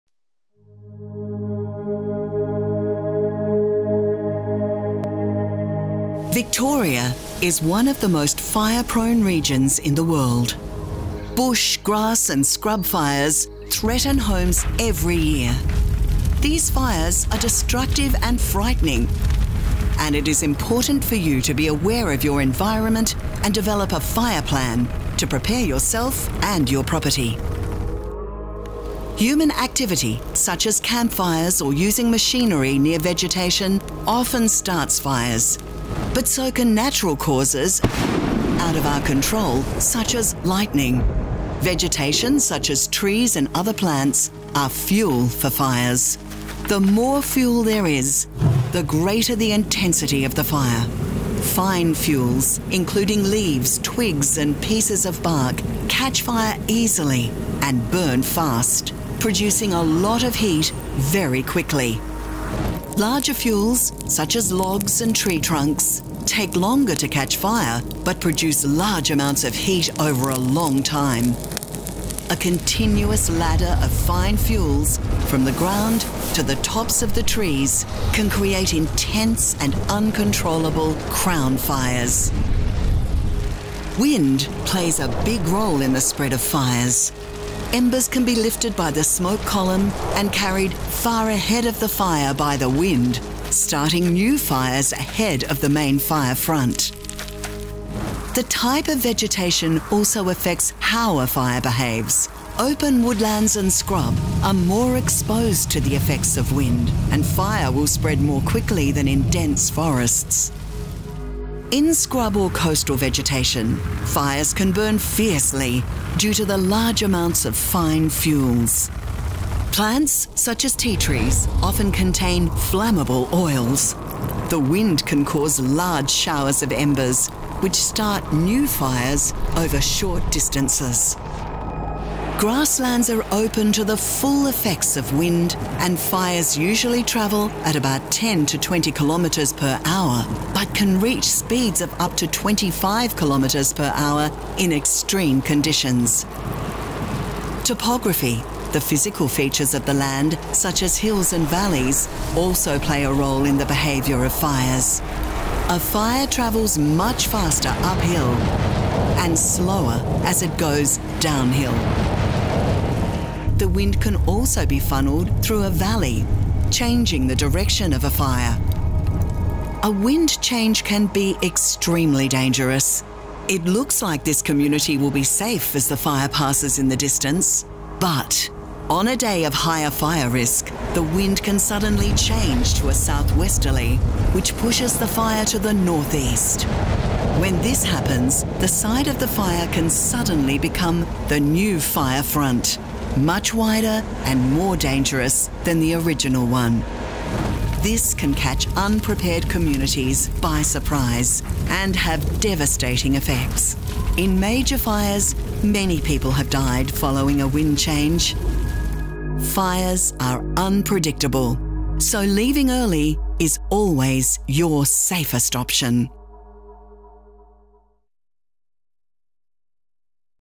Bushfire Behaviour
It is important our communities have some understanding of basic fire behaviour so they can understand the reasons for actions taken in planning and preparing for fire.
This animation covers the three major factors influencing bushfire behaviour - vegetation, weather and topography.
The way bushfire behaves can directly influence the decisions you need to make in planning and preparing yourself and your property for bushfire.

Owner: Community Preparedness, LM
Uploaded by: C&SR, MA